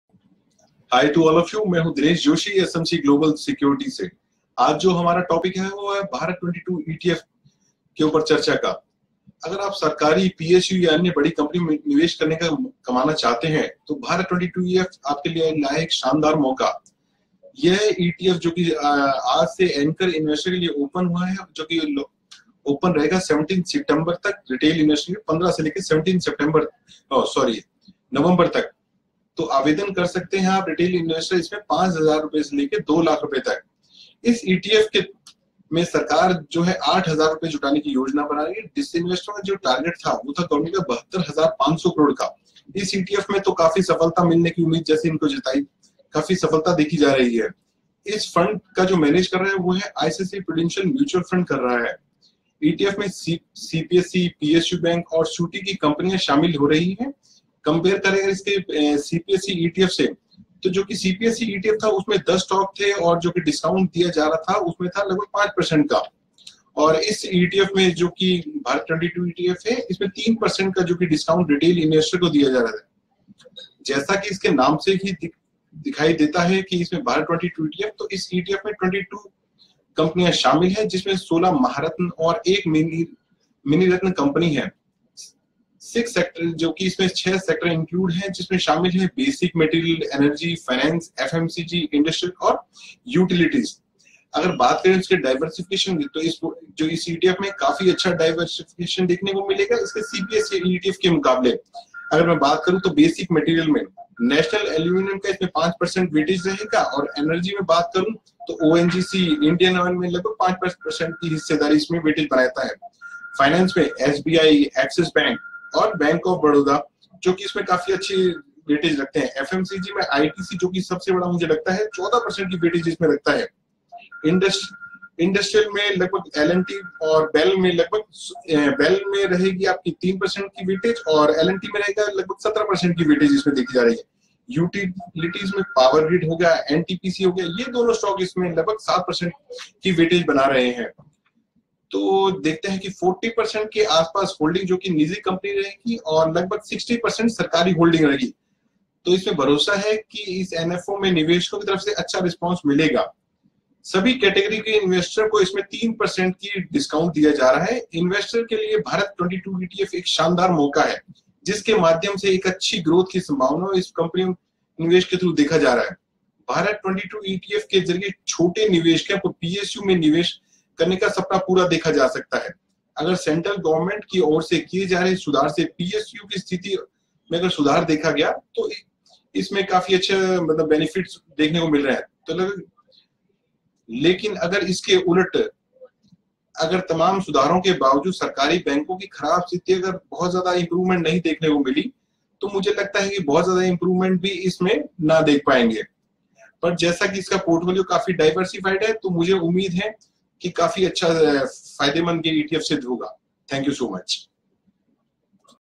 "Bharat 22 ETF Review" by SMC Research Expert Mr. Dinesh Joshi
Get to know "Bharat 22-ETF Review" by SMC Research Expert Mr. Dinesh Joshi(Sr. Research Analyst,Equity(Fundamental)).